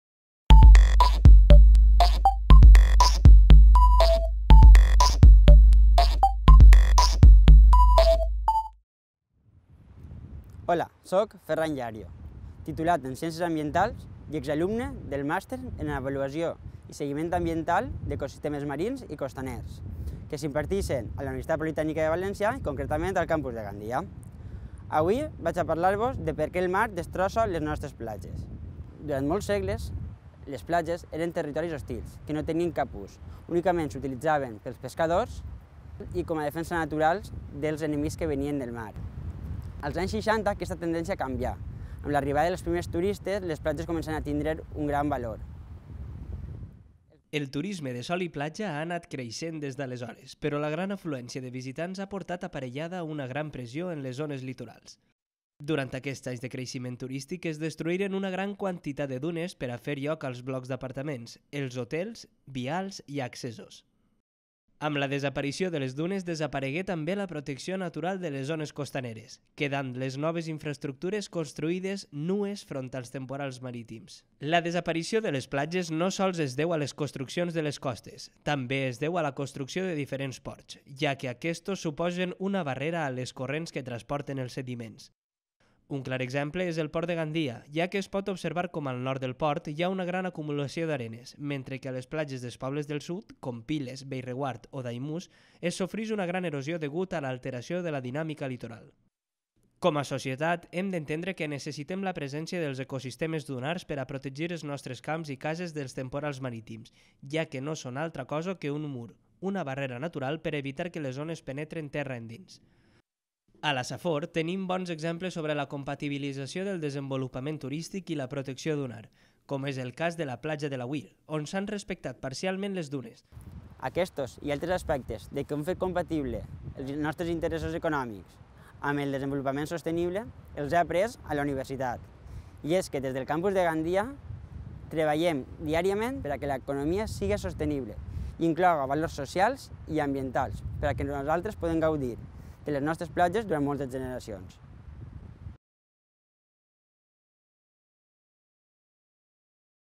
La importància de les dunes - Ferran Llario
Per què cal omplir les platges del sud de Gandia amb arena? Ferran Llario Sempere, graduat en Ciències Ambientals, ens explica la importància de les dunes.
+ info sobre el Màster en Avaluació i Seguiment Ambiental d'Ecosistemes Marins i Costaners